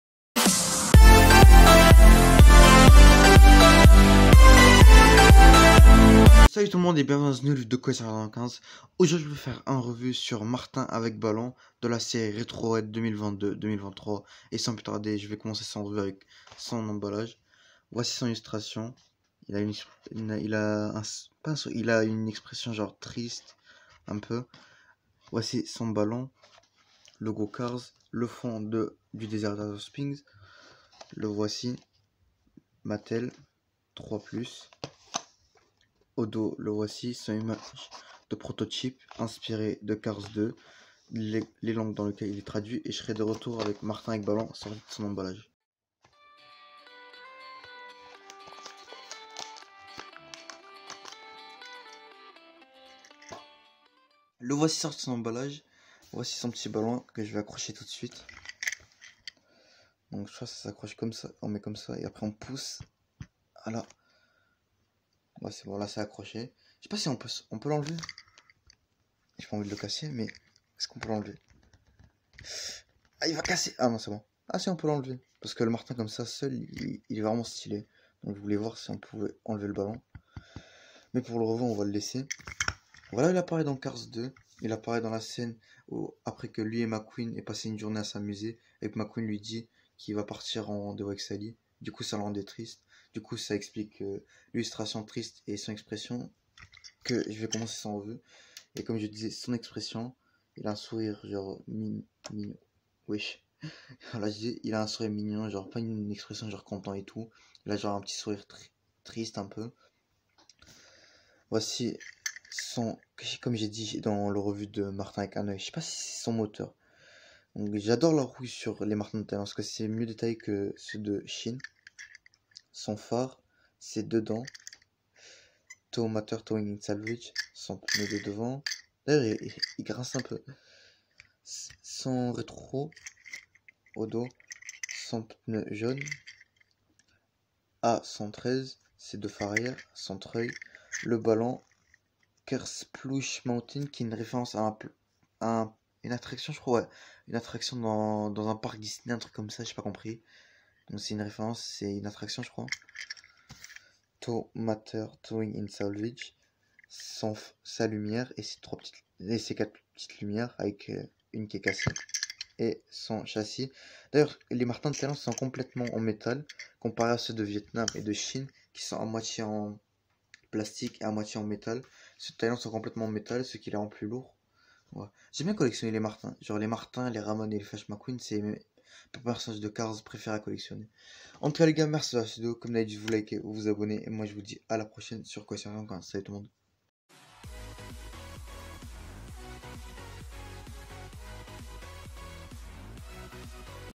Martin avec Ballon (revue) (séries "retro red" 2022/23)
yo salut tout le monde et bienvenue dans une nouvelle vidéo de Collectionneur 95 aujourd'hui je vais faire un revue sur Martin avec Ballon